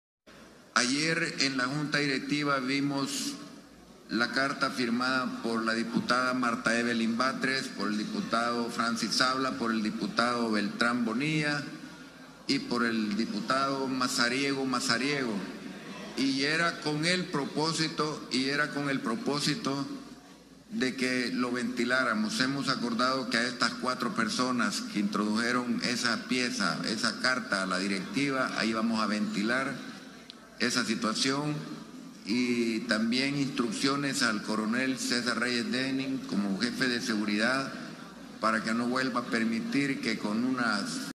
Pleito entre el diputado Zablah y Shafik Handal
El diputado de GANA Francis Zablah junto a al diputado del FMLN Jorge Shafik Handal, protagonizaron un encontronazo luego que Zablah informara que iría a la FGR a presentar aviso por amenazas recibidas tras los disturbios del martes afuera de la comisión de medio ambiente en la Asamblea